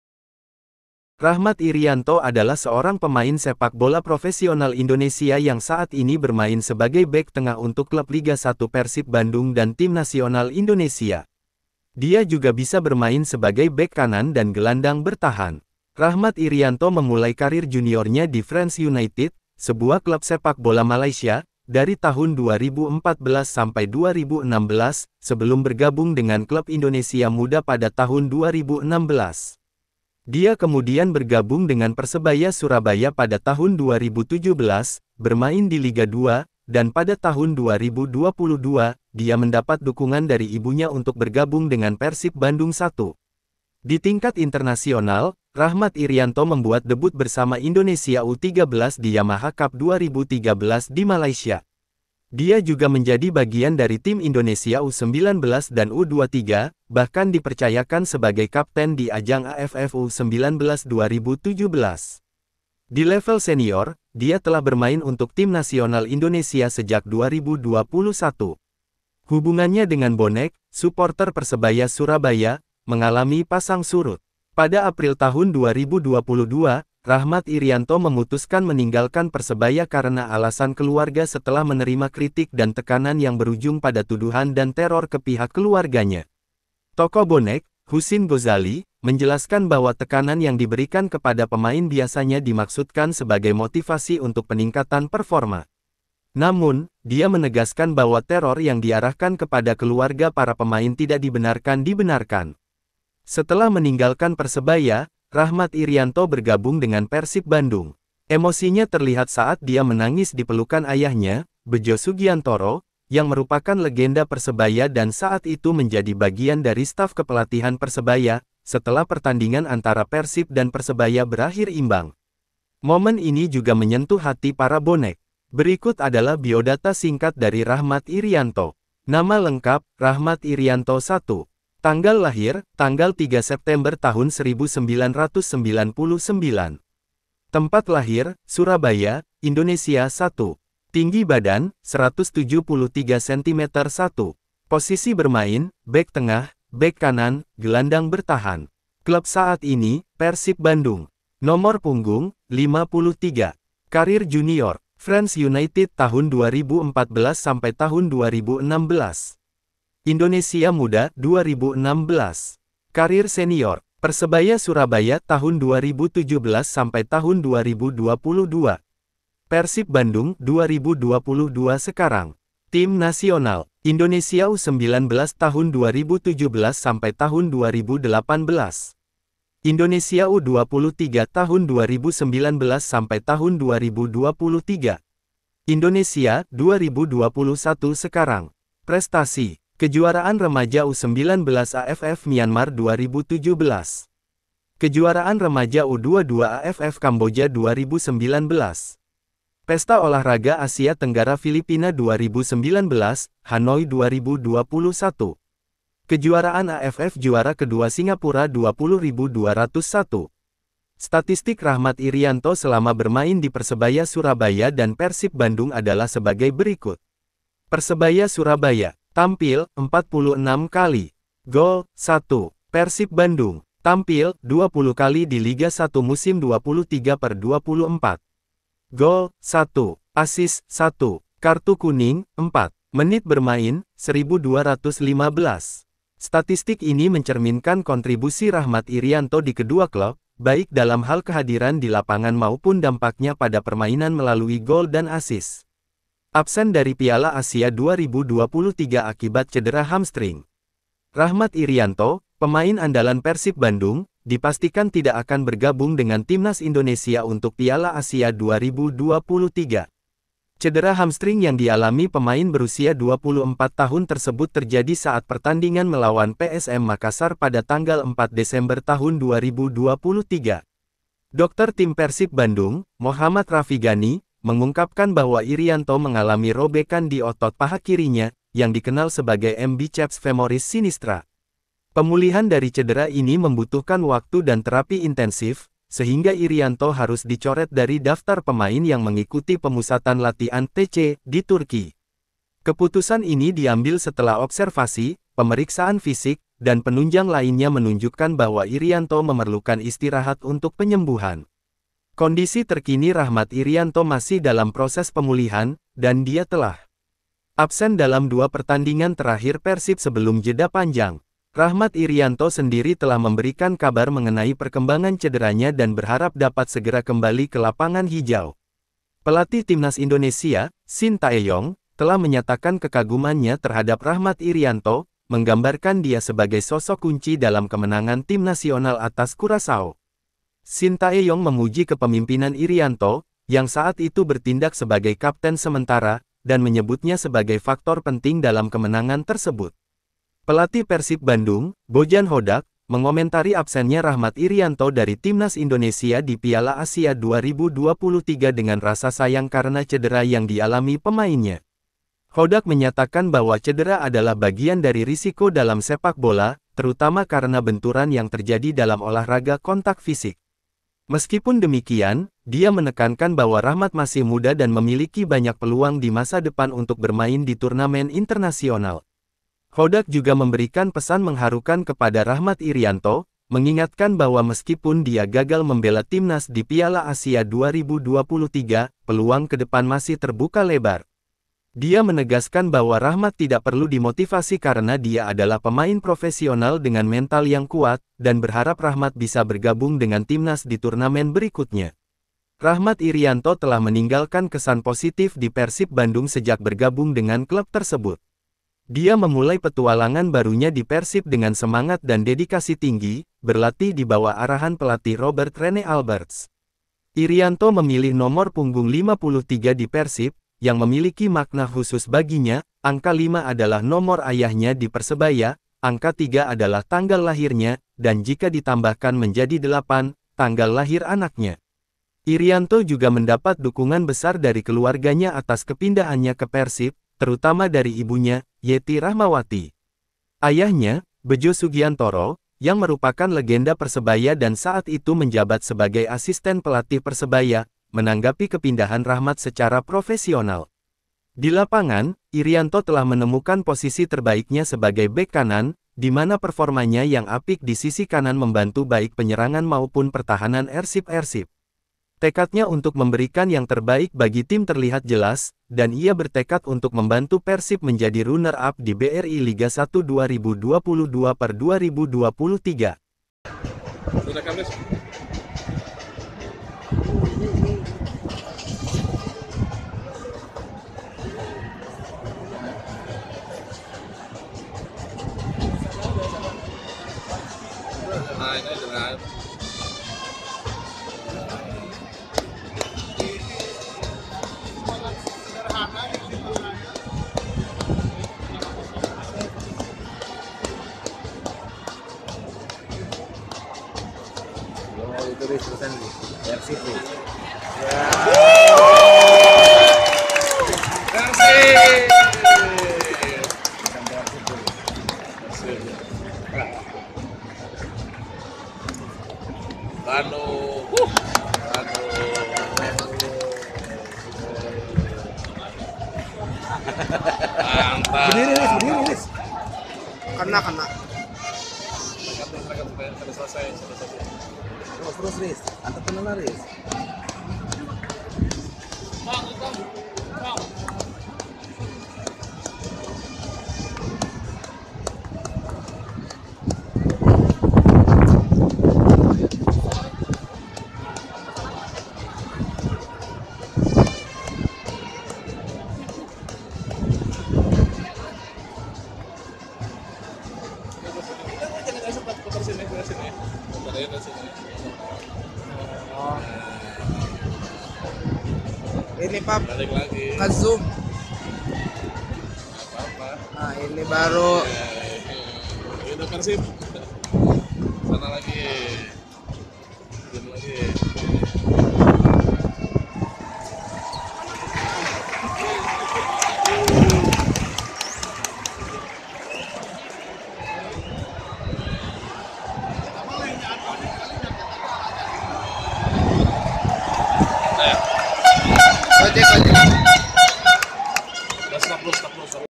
RAHMAT IRIANTO - PERSIB BANDUNG- MASIH KASUAT SUAT KANU HEJO ❤❤😀🙏
Rahmat Irianto adalah seorang pemain sepak bola profesional Indonesia yang saat ini bermain sebagai bek tengah untuk klub Liga 1 Persib Bandung dan Tim nasional Indonesia. Dia juga bisa bermain sebagai bek kanan dan gelandang bertahan. Rahmat Irianto memulai karir juniornya di Frenz United, sebuah klub sepak bola Malaysia, dari tahun 2014 sampai 2016, sebelum bergabung dengan klub Indonesia Muda pada tahun 2016. Dia kemudian bergabung dengan Persebaya Surabaya pada tahun 2017, bermain di Liga 2, dan pada tahun 2022, dia mendapat dukungan dari ibunya untuk bergabung dengan Persib Bandung1.

Di tingkat internasional, Rahmat Irianto membuat debut bersama Indonesia U-13 di Yamaha Cup 2013 di Malaysia. Dia juga menjadi bagian dari tim Indonesia U-19 dan U-23, bahkan dipercayakan sebagai kapten di ajang AFF U-19 2017. Di level senior, dia telah bermain untuk Tim nasional Indonesia sejak 2021.

Hubungannya dengan Bonek, suporter Persebaya Surabaya, mengalami pasang surut. Pada April 2022, Rahmat Irianto memutuskan meninggalkan Persebaya karena alasan keluarga setelah menerima kritik dan tekanan yang berujung pada tuduhan dan teror ke pihak keluarganya.

Tokoh Bonek, Husin Ghozali, menjelaskan bahwa tekanan yang diberikan kepada pemain biasanya dimaksudkan sebagai motivasi untuk peningkatan performa. Namun, dia menegaskan bahwa teror yang diarahkan kepada keluarga para pemain tidak dibenarkan2.

Setelah meninggalkan Persebaya, Rahmat Irianto bergabung dengan Persib Bandung.

Emosinya terlihat saat dia menangis di pelukan ayahnya, Bejo Sugiantoro, yang merupakan legenda Persebaya dan saat itu menjadi bagian dari staf kepelatihan Persebaya, setelah pertandingan antara Persib dan Persebaya berakhir imbang. Momen ini juga menyentuh hati para Bonek.

Berikut adalah biodata singkat dari Rahmat Irianto:

Nama Lengkap: Rachmat Irianto1

Tanggal Lahir: 3 September 1999.

Tempat Lahir: Surabaya, Indonesia1

Tinggi Badan: 173 cm1

Posisi Bermain: Bek tengah, bek kanan, gelandang bertahan.

Klub Saat Ini: Persib Bandung.

Nomor Punggung: 53.

Karier Junior:

Frenz United (2014–2016)

Indonesia Muda (2016)

Karier Senior:

Persebaya Surabaya (2017–2022)

Persib Bandung (2022–Sekarang)

Tim Nasional:

Indonesia U19 (2017–2018)

Indonesia U23 (2019–2023)

Indonesia (2021–Sekarang)

Prestasi:

Kejuaraan Remaja U-19 AFF Myanmar 2017

Kejuaraan Remaja U-22 AFF Kamboja 2019

Pesta Olahraga Asia Tenggara Filipina 2019, Hanoi 2021

Kejuaraan AFF Juara kedua Singapura 20201

Statistik Rahmat Irianto selama bermain di Persebaya Surabaya dan Persib Bandung adalah sebagai berikut:

Persebaya Surabaya:

Tampil: 46 kali

Gol: 1

Persib Bandung:

Tampil: 20 kali di Liga 1 musim 23/24

Gol: 1

Assist: 1

Kartu Kuning: 4

Menit Bermain: 1.215’.

Statistik ini mencerminkan kontribusi Rahmat Irianto di kedua klub, baik dalam hal kehadiran di lapangan maupun dampaknya pada permainan melalui gol dan assist.

Absen dari Piala Asia 2023 Akibat Cedera Hamstring

Rahmat Irianto, pemain andalan Persib Bandung, dipastikan tidak akan bergabung dengan Timnas Indonesia untuk Piala Asia 2023. Cedera hamstring yang dialami pemain berusia 24 tahun tersebut terjadi saat pertandingan melawan PSM Makassar pada tanggal 4 Desember 2023. Dokter tim Persib Bandung, Mochamad Rafi Ghani, mengungkapkan bahwa Irianto mengalami robekan di otot paha kirinya, yang dikenal sebagai M biceps femoris sinistra.

Pemulihan dari cedera ini membutuhkan waktu dan terapi intensif, sehingga Irianto harus dicoret dari daftar pemain yang mengikuti pemusatan latihan (TC) di Turki. Keputusan ini diambil setelah observasi, pemeriksaan fisik, dan penunjang lainnya menunjukkan bahwa Irianto memerlukan istirahat untuk penyembuhan.

Kondisi terkini Rahmat Irianto masih dalam proses pemulihan, dan dia telah absen dalam dua pertandingan terakhir Persib sebelum jeda panjang. Rahmat Irianto sendiri telah memberikan kabar mengenai perkembangan cederanya dan berharap dapat segera kembali ke lapangan hijau.

Pelatih Timnas Indonesia, Shin Tae-yong, telah menyatakan kekagumannya terhadap Rahmat Irianto, menggambarkan dia sebagai sosok kunci dalam kemenangan tim nasional atas Curacao.

Shin Tae-yong memuji kepemimpinan Irianto, yang saat itu bertindak sebagai kapten sementara, dan menyebutnya sebagai faktor penting dalam kemenangan tersebut.

Pelatih Persib Bandung, Bojan Hodak, mengomentari absennya Rahmat Irianto dari Timnas Indonesia di Piala Asia 2023 dengan rasa sayang karena cedera yang dialami pemainnya. Hodak menyatakan bahwa cedera adalah bagian dari risiko dalam sepak bola, terutama karena benturan yang terjadi dalam olahraga kontak fisik. Meskipun demikian, dia menekankan bahwa Rahmat masih muda dan memiliki banyak peluang di masa depan untuk bermain di turnamen internasional.